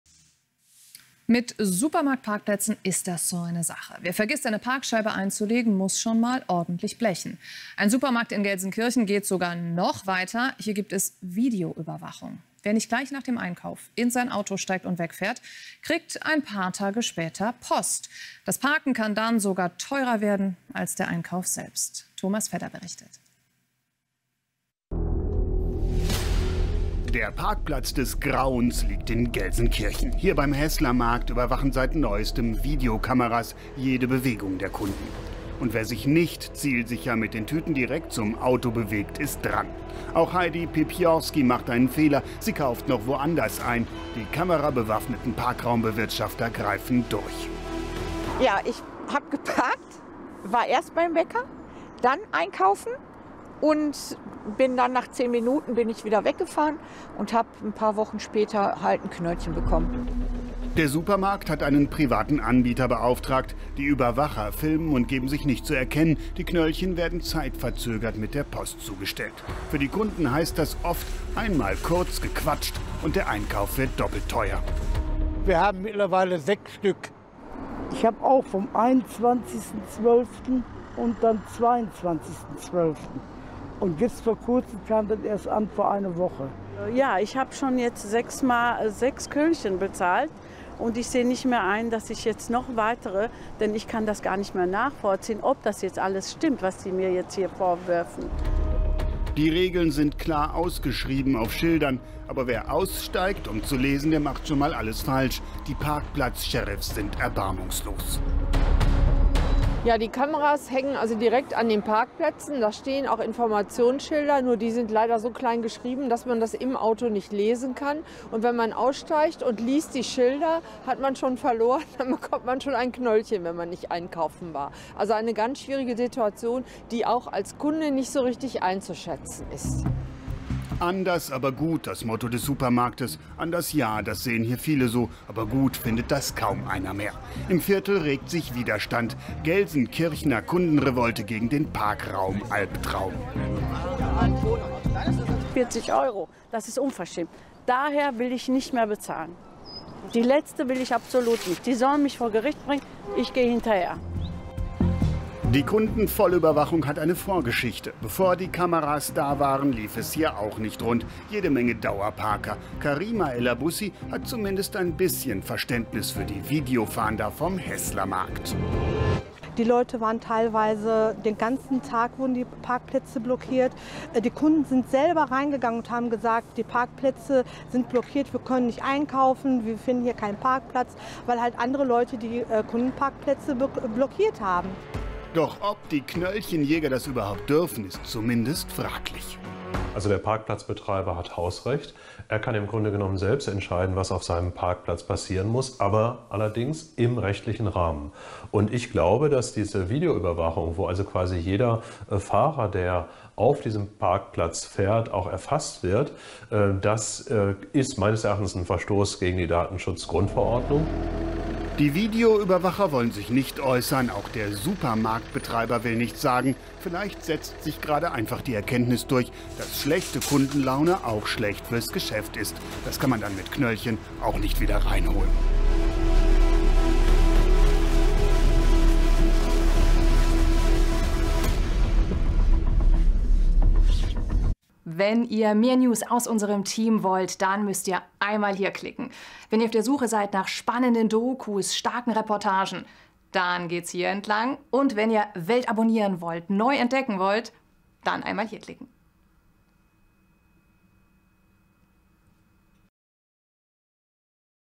PARKPLATZ-STRESS: Ein Gefühl von Abzocke am Supermarkt macht sich breit
Beim Einkauf im Supermarkt müssen die Kunden jetzt aufpassen. Denn immer mehr Supermarkt-Parkplätze werden jetzt von Parkraumbewirtschaftern per Video überwacht. Wer neben dem Einkauf von Lebensmitteln auch noch zur benachbarten Apotheke geht, riskiert ein saftiges Knöllchen. Die Kunden sind empört.